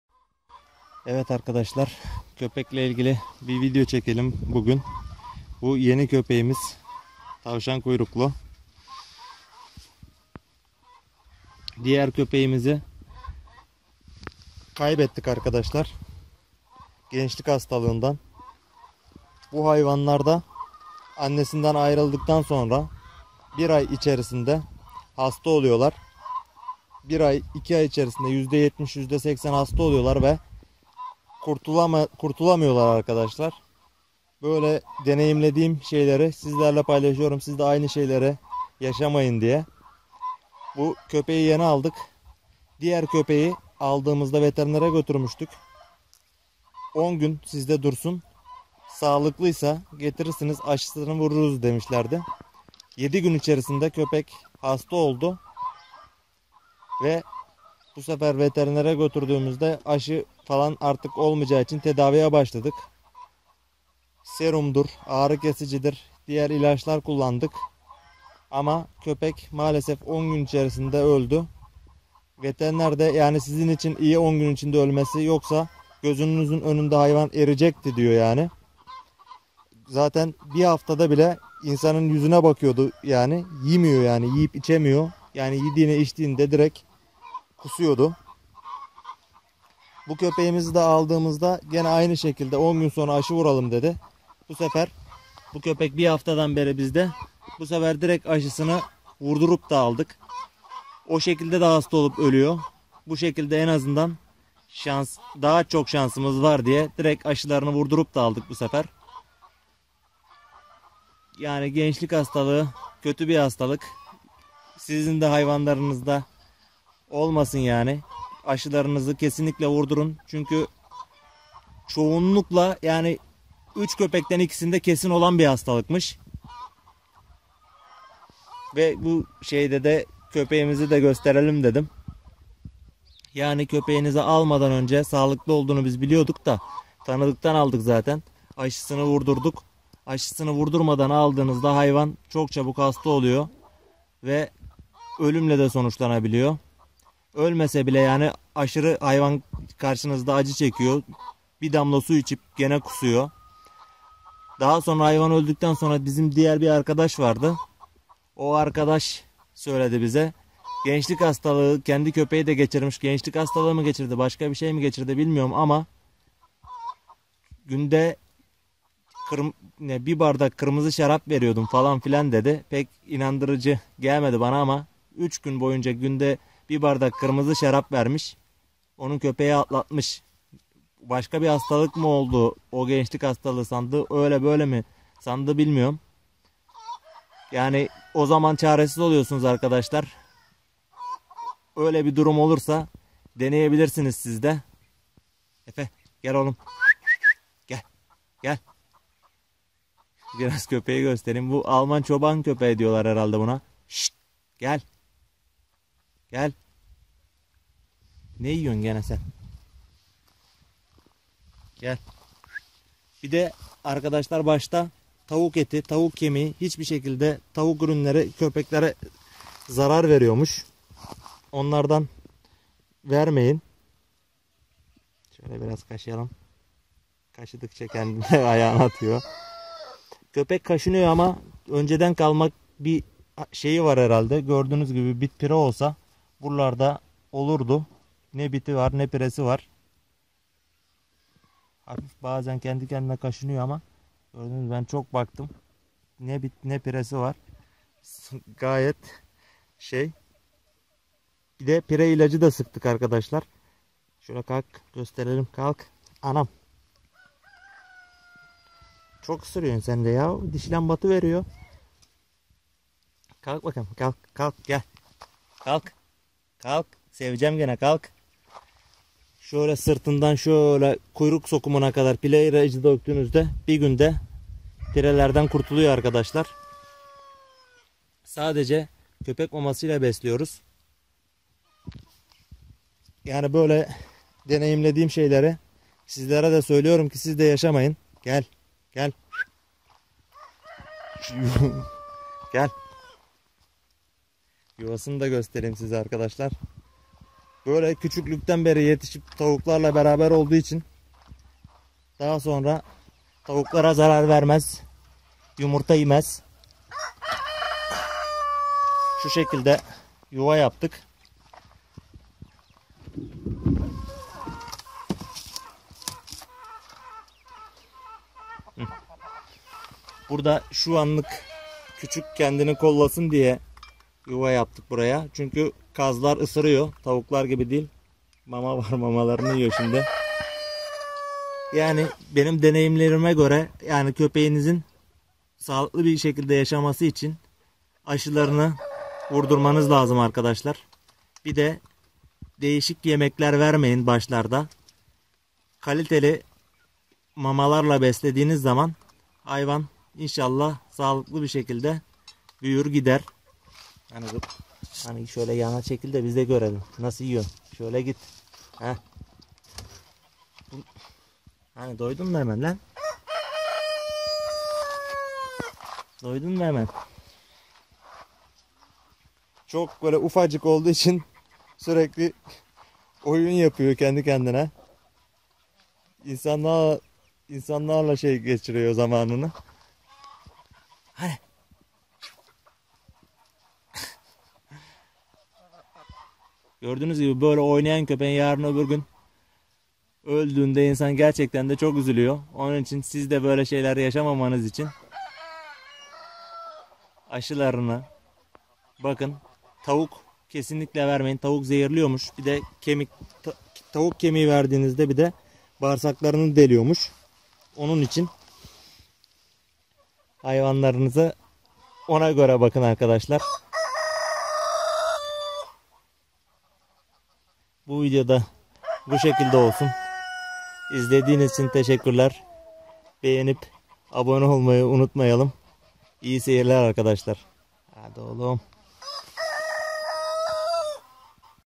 Köpeklerde Gençlik Hastalığı
Evet arkadaşlar bugün sizlere köpeklerde gençlik hastalığını anlattım.
İyi seyirler.